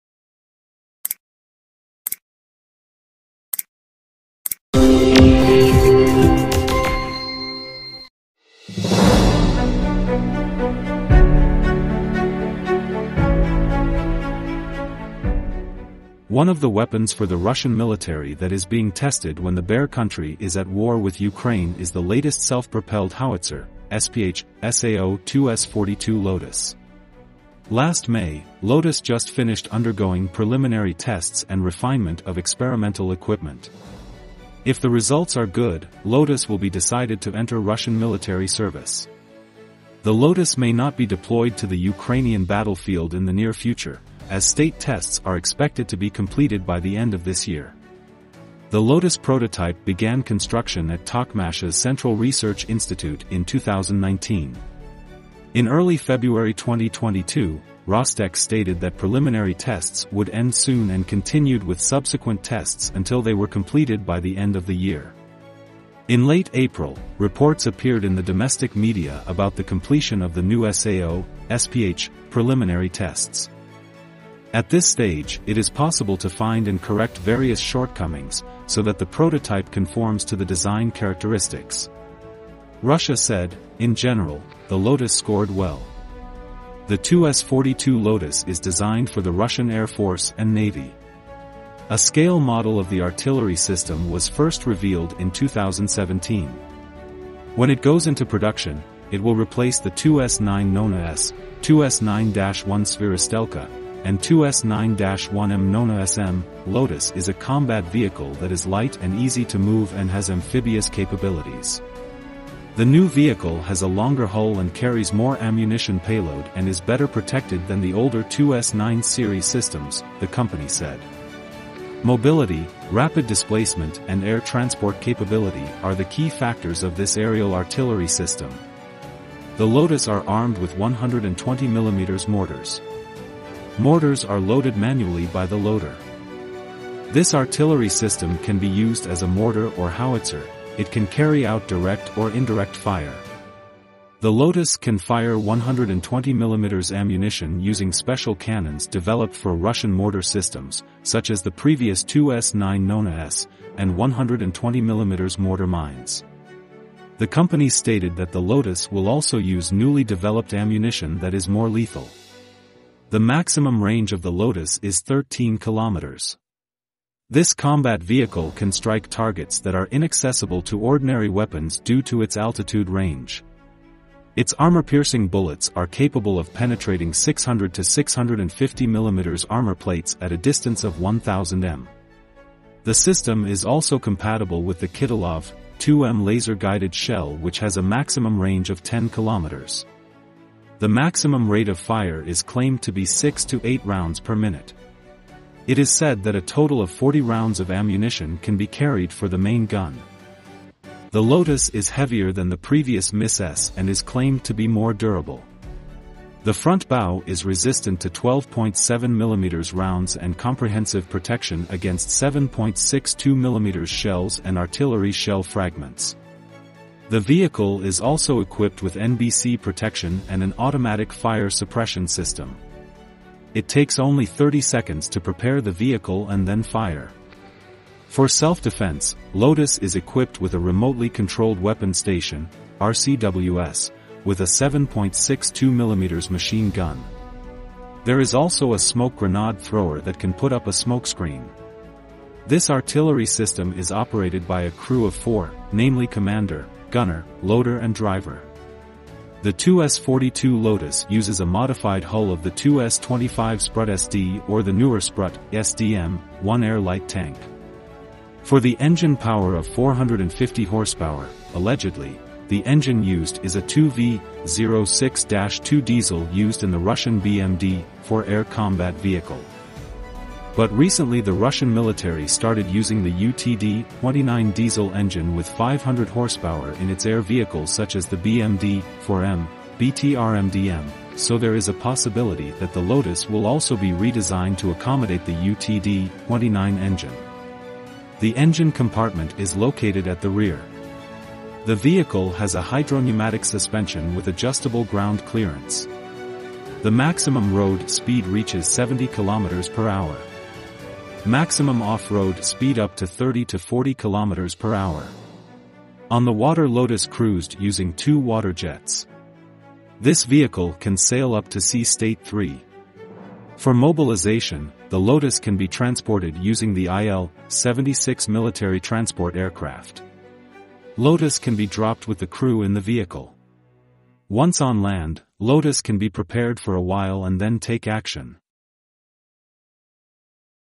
The latest amphibious SPH for the Russian military?
One of the weapons for the Russian military that is being tested when the Bear Country is at war with Ukraine is the latest self-propelled howitzer (SPH) SAO 2S42 Lotos. Last May, Lotos just finished undergoing preliminary tests and refinement of experimental equipment. If the results are good, Lotos will be decided to enter Russian military service.